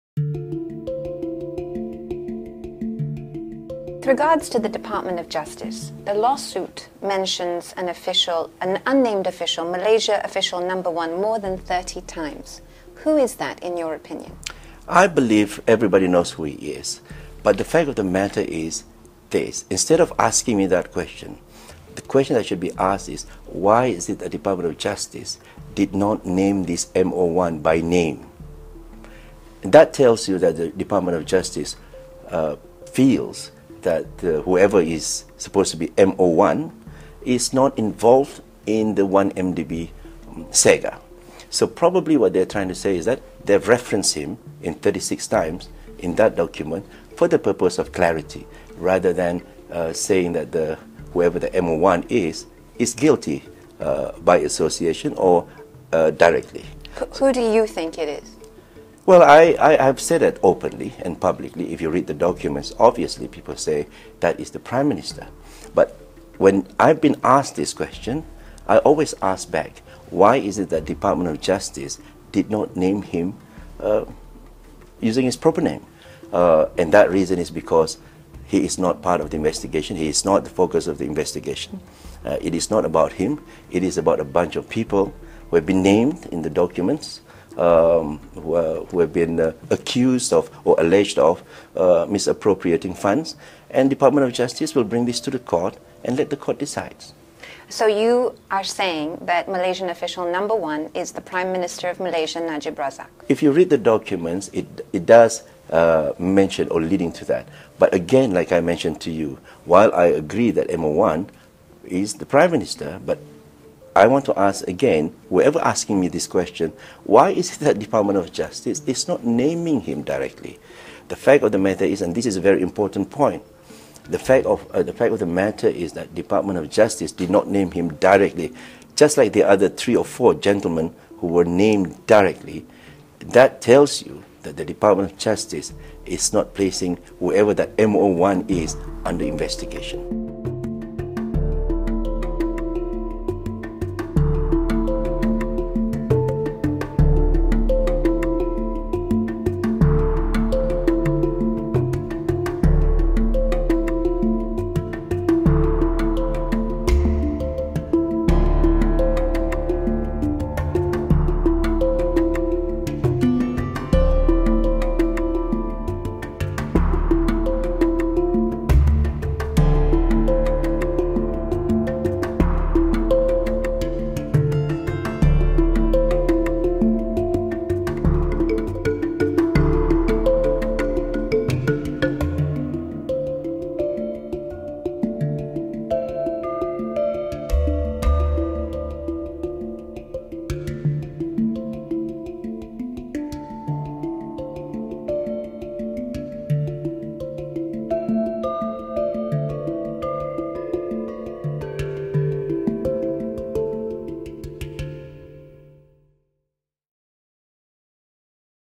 Reactions after Rahman Dahlan tells BBC that Najib is MO1
Reactions from political figures after minister Datuk Seri Abdul Rahman Dahlan admits to BBC Asia business correspondent Karishma Vaswani that 'Malaysian Official 1' is the PM, Datuk Seri Najib Razak.